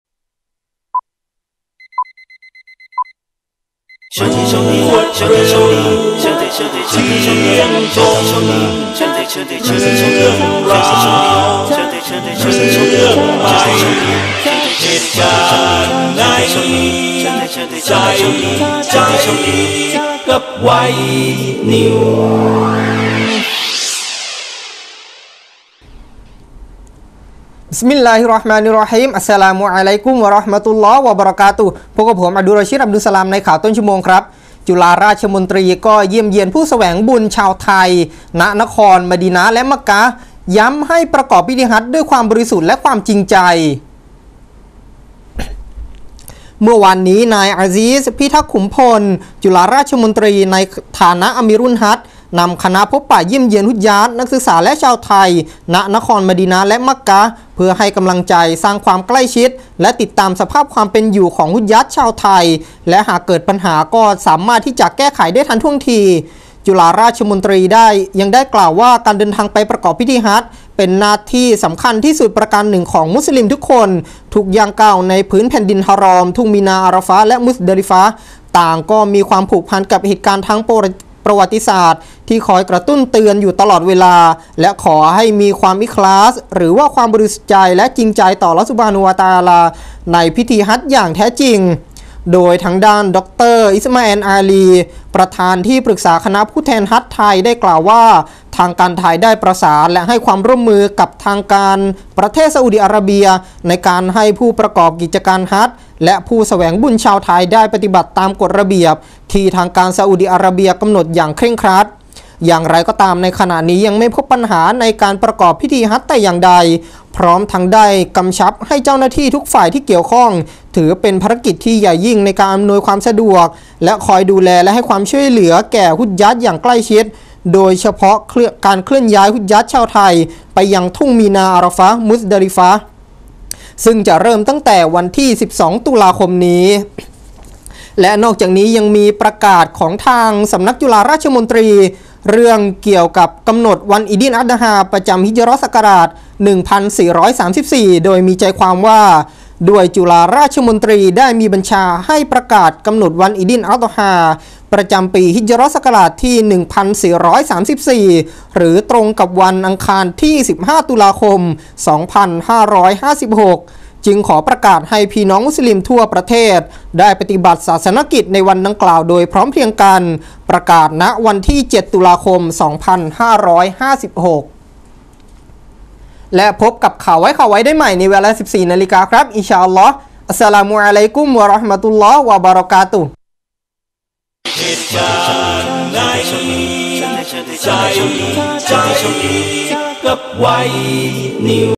จุฬาราชมนตรีเยี่ยมฮุจญาตไทย
จุฬาราชมนตรีเยี่ยมเยียนผู้แสวงบุญและพบปะชาวไทย ณ นครมาดีนะห์และนครมักกะห์ ย้ำจงประกอบพิธีฮัจย์ด้วยความบริสุทธิ์ใจและจริงใจ

เมื่อวานี้นายอาศิส พิทักษ์คุมพล จุฬาราชมนตรี ในฐานะอมีรุลฮัจญ์นำคณะพบปะเยี่ยมเยียนฮุจญาต นักศึกษาและชาวไทย ณ นครมะดีนะห์ และมักกะห์ เพื่อให้กำลังใจ สร้างความใกล้ชิด และติดตามสภาพความเป็นอยู่ฮุจญาตชาวไทย และหากเกิดปัญหาสามารถแก้ไขได้ทันท่วงที

จุฬาราชมนตรี กล่าวว่า การเดินทางไปประกอบพิธีฮัจย์เป็นหน้าที่สำคัญที่สุดประการหนึ่งของมุสลิมทุกคน ทุกย่างก้าวในพื้นแผ่นดินฮารอม ทุ่งมีนา อารอฟะฮ์ และมุซดาลีฟะฮ์ ต่างมีความผูกพันกับเหตุการณ์ทางประวัติศาสตร์ที่คอยกระตุ้นเตือนอยู่ตลอดเวลา และขอให้มีความอิคลาศบริสุทธิ์ใจและจริงใจต่ออัลลอฮ์ในพิธีฮัจย์อย่างแท้จริง

ดร.อิสมาแอล อาลี ประธานที่ปรึกษาคณะผู้แทนฮัจญ์ไทยกล่าวว่า ทางการไทยได้ประสานและให้ความร่วมมือกับทางการประเทศซาอุดิอาระเบีย ในการให้ผู้ประกอบการกิจการฮัจย์และผู้แสวงบุญชาวไทยได้ปฏิบัติตามกฏระเบียบที่ทางการซาอุดิอาระเบียกำหนดอย่างเคร่งครัด

อย่างไรก็ตามในขณะนี้ยังไม่พบปัญหาในการประกอบพิธีฮัจย์แต่อย่างใด พร้อมทั้งได้กำชับให้เจ้าหน้าที่ทุกฝ่ายที่เกี่ยวข้อง ถือเป็นภารกิจที่ยิ่งใหญ่ ในการอำนวยความสะดวกคอยดูแลและให้ความช่าวเหลือแก่ฮุจญาตอย่างใกล้ชิด โดยเฉพาะการเคลื่อนย้ายฮุจญาตชาวไทยเพื่อไปยัง ทุ่งมีนา อารอฟะฮ์และ มุซดาลีฟะฮ์ ซึ่งจะเริ่มตั้งวันที่ 12 ตุลาคมนี้